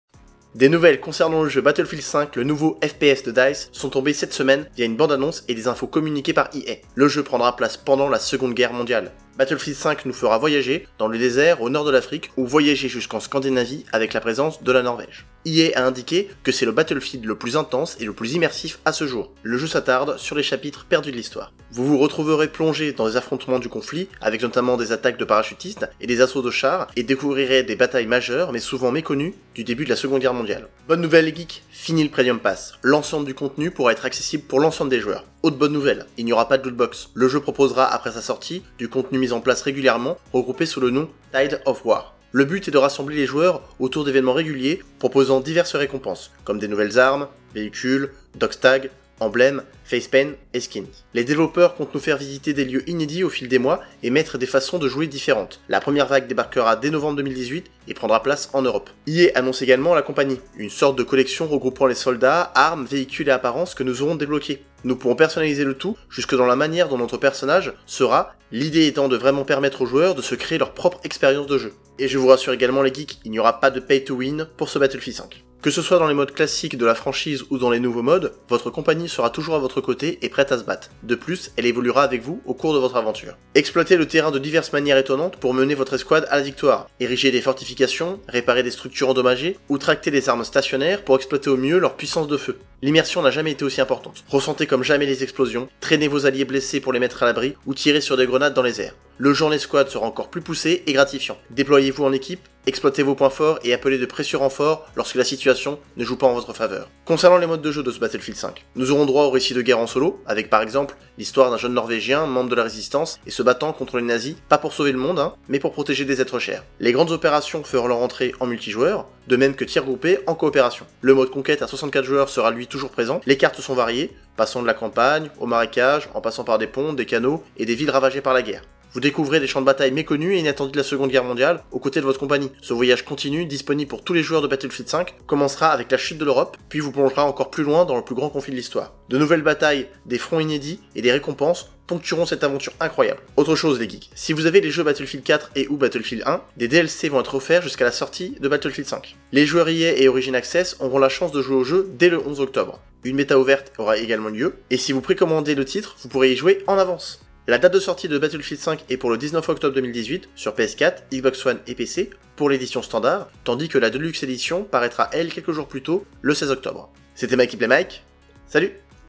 Toutes les NOUVEAUTÉS de BATTLEFIELD V !
EA vient de dévoiler toutes les nouveautés du jeu Battlefield V !

► News, tests, reviews, VLOGS, théories, etc ... Retrouve sur cette chaîne toute l'actualité geek décryptée en vidéos !

► Voix et Texte de Mykimic / Montage de Matteo Sapin